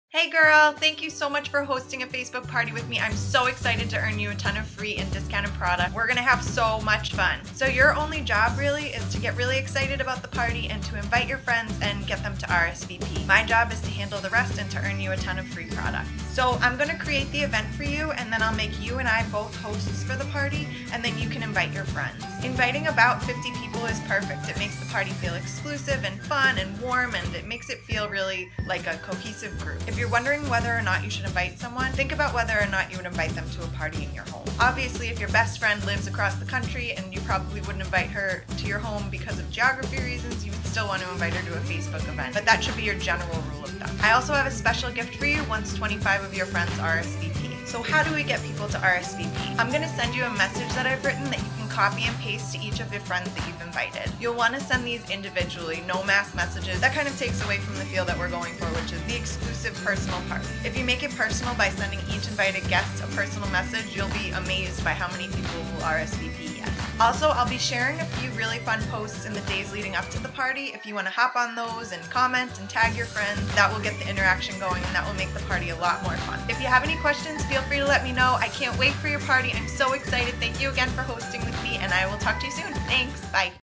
Thank you for Hosting a Party With Me!
Thank you so much for hosting a party with me!  Here are some tips to maximize your party's success!  :)